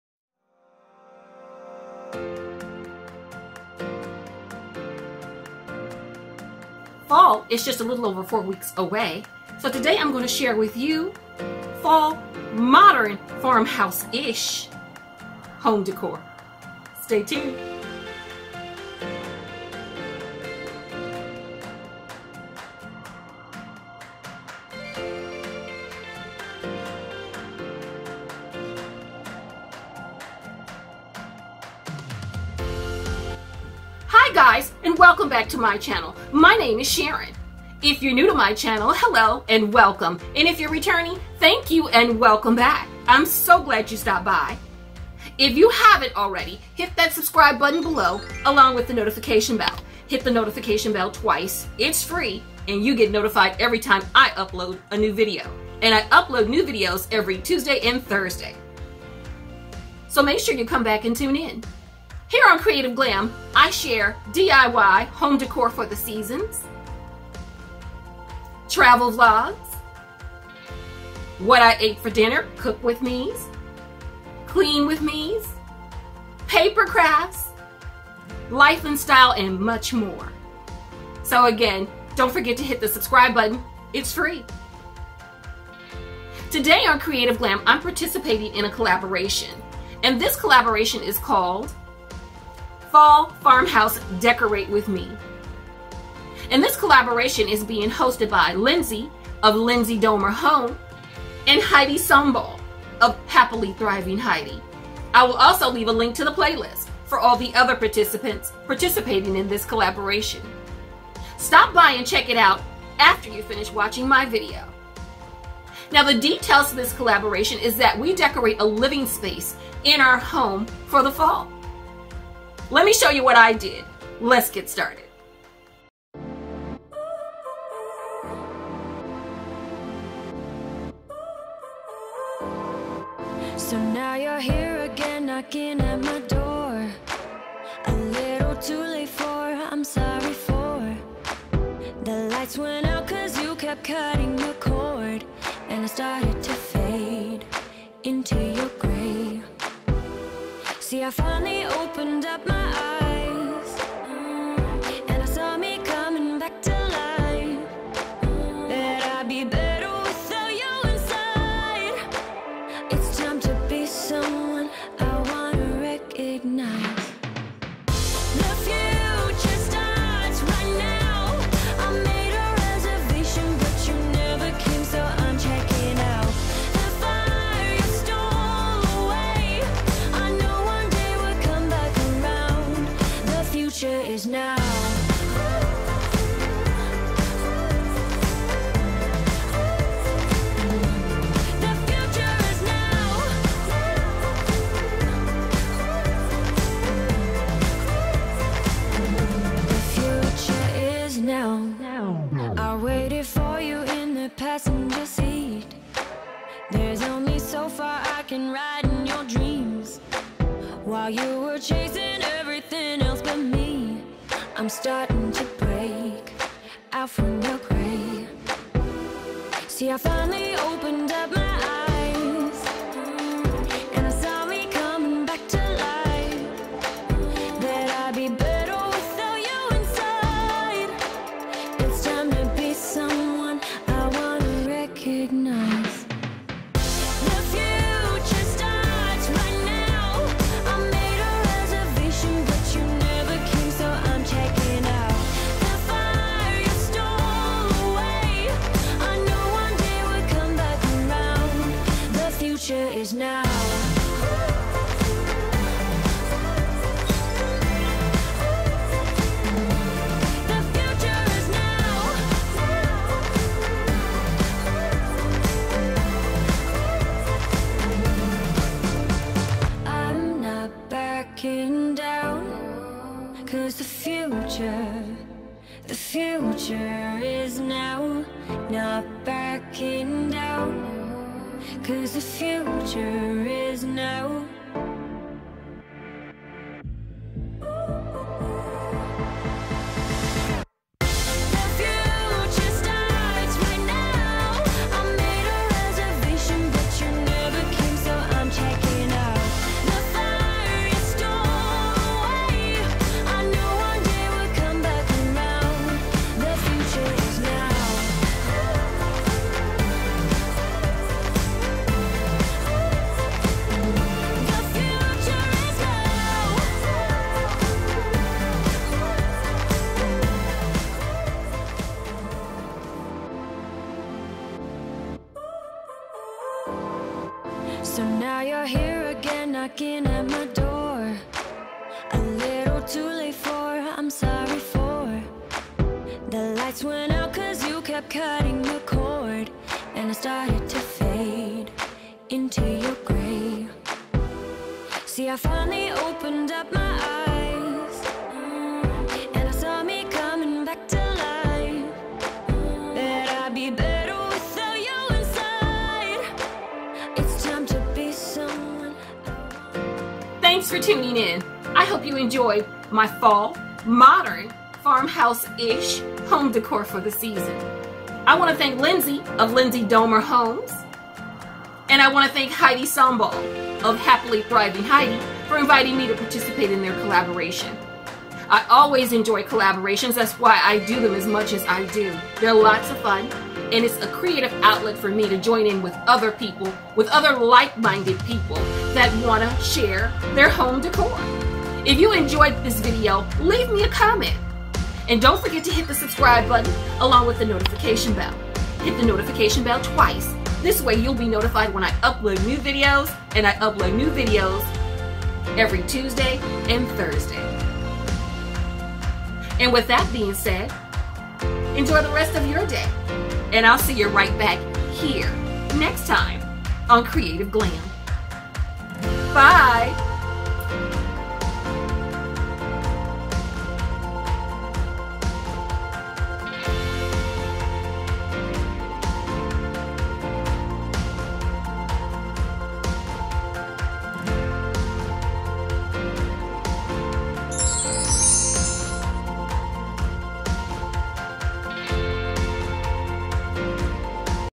Fall Farmhouse Decorate With Me || Autumn Decor Ideas
Hi Guys! 💖 Today I am participating in a collaboration called: Fall Farmhouse Decorate With Me || Fall Decor Ideas  I hope you enjoy. I love What I Do and I Do What I Love. 💖

❤ Lighting Kit